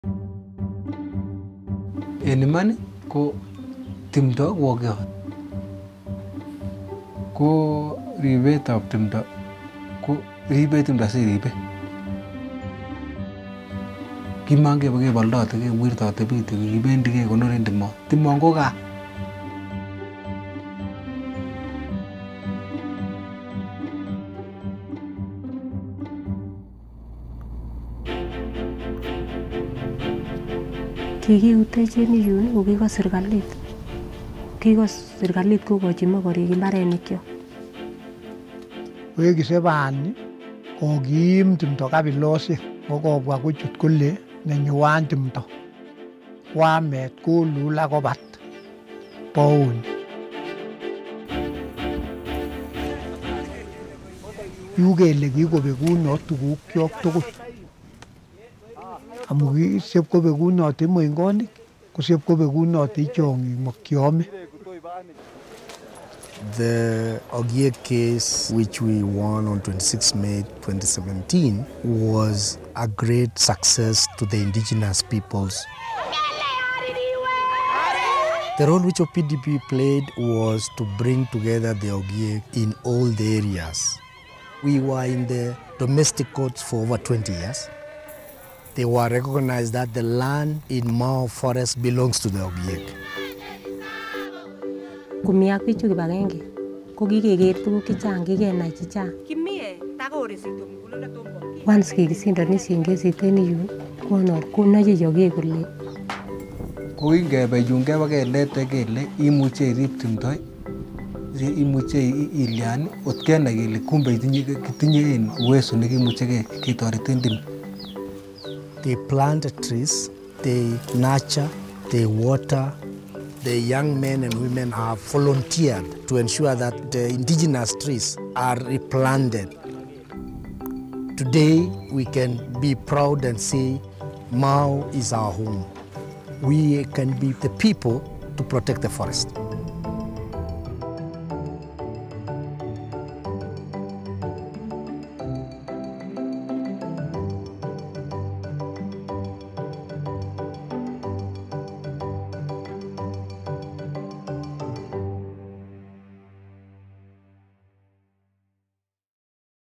Hope Grows: Kenya’s Ogiek Defend Their Forests
The Ogiek people are an indigenous group in Kenya whose home, the ancient Mau Forest, was being razed by loggers who care more for profits than the planet. Learn how the Ogiek are saving their forest while protecting the environment and their culture!

Want more stories of change from around the globe?